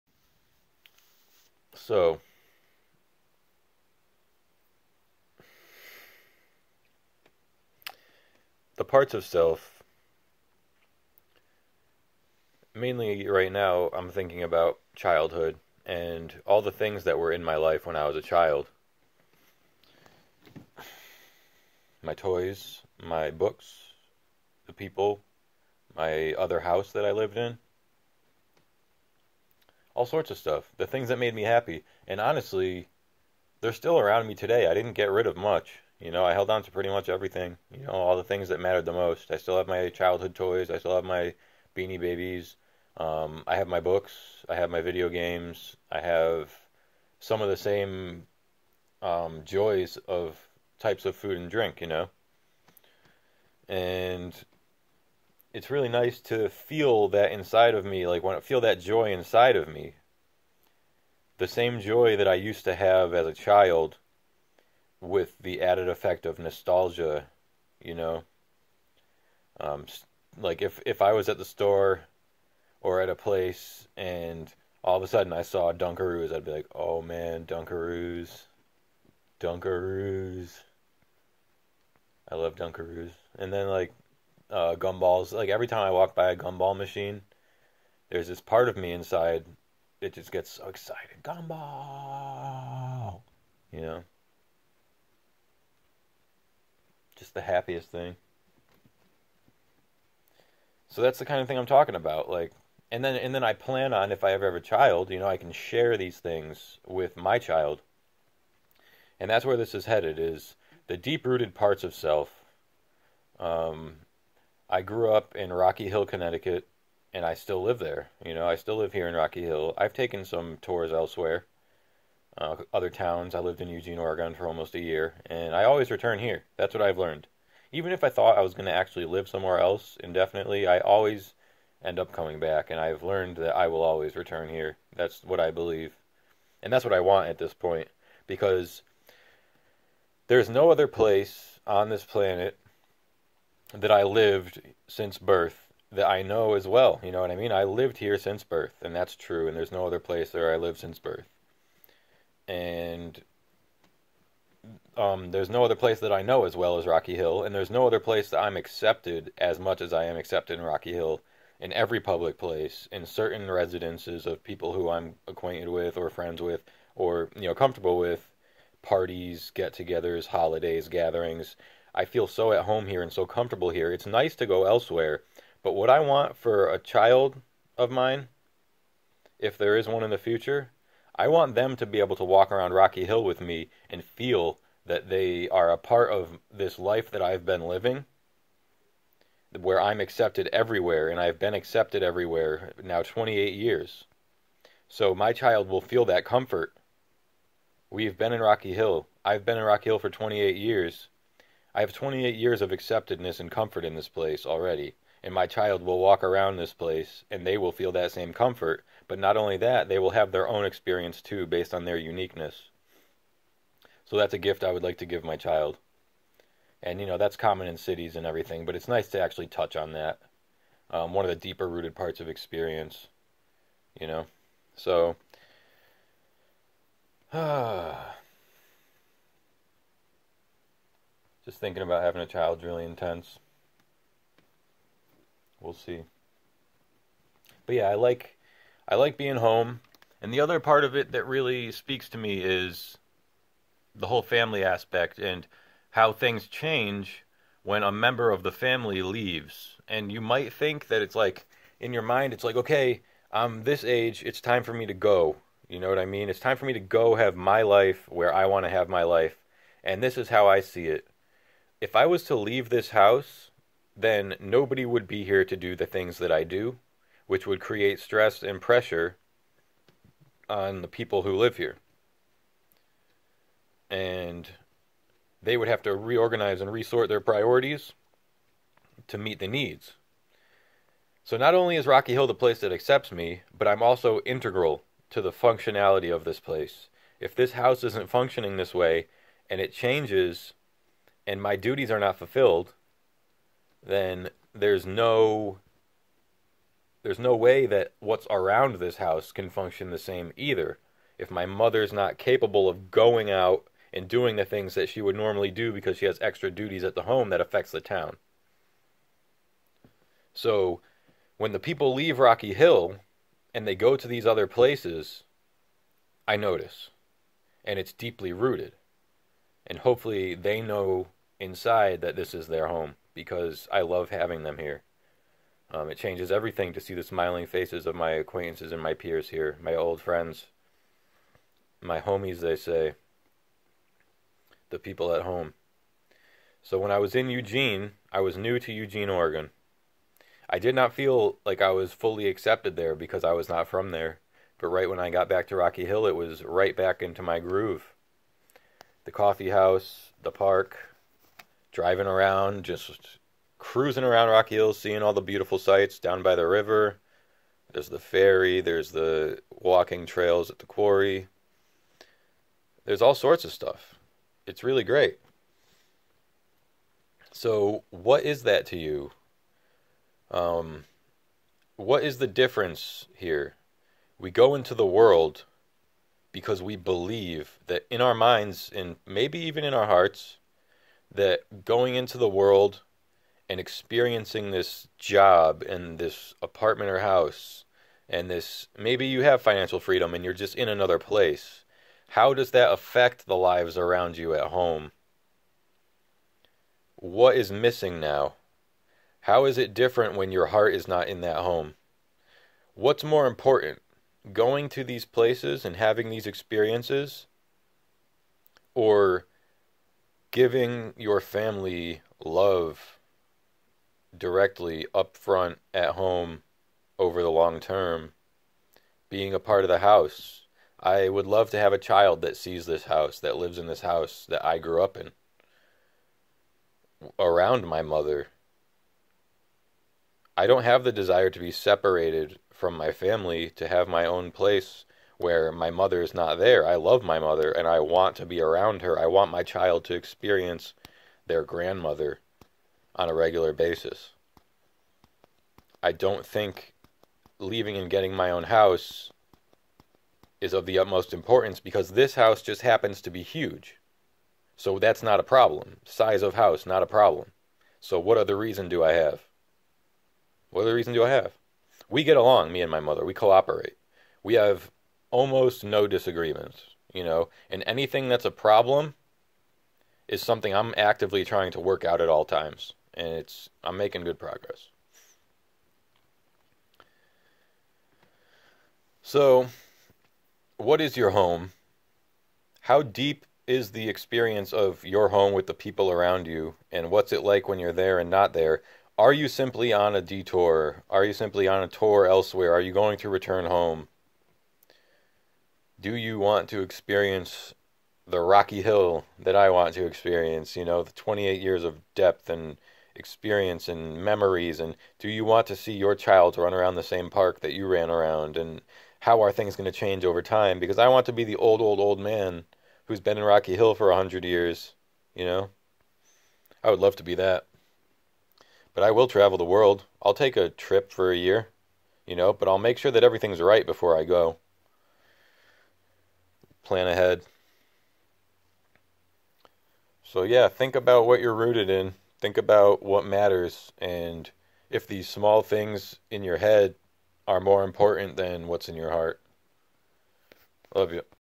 Priorities And Experiential Depth
Why would life have to be that sad because of someone leaving by choice? Is home more important? Is it worth leaving knowing that they are going to be without your love and support and you without theirs? Hometown hero!!